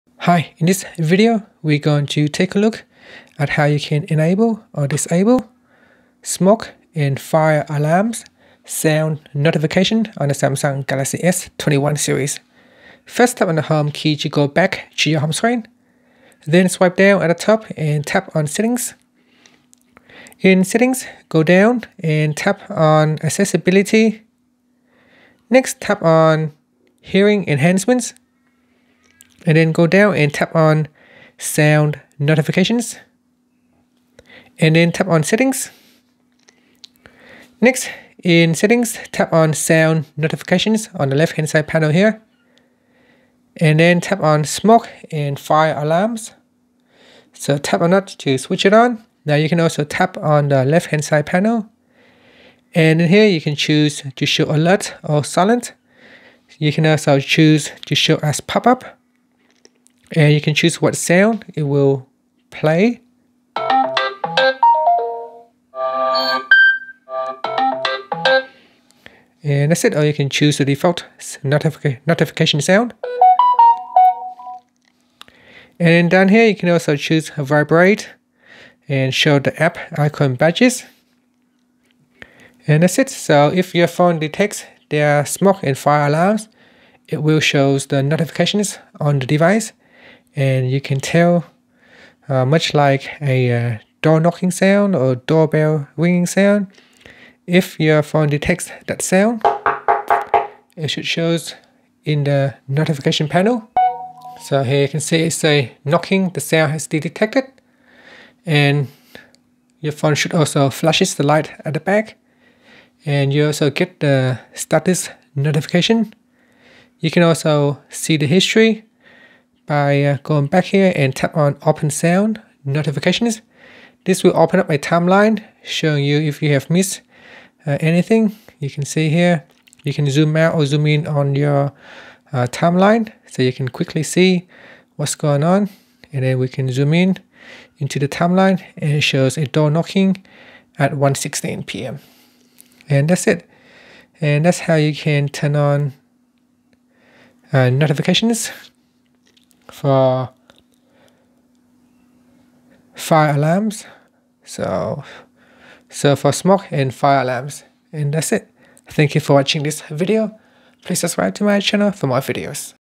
Galaxy S21/Ultra/Plus: How to Enable/Disable Smoke & Fire Alarms Sound Notifications
Learn how you can enable or disable smoke & fire alarm sound notifications on the Galaxy S21/Ultra/Plus.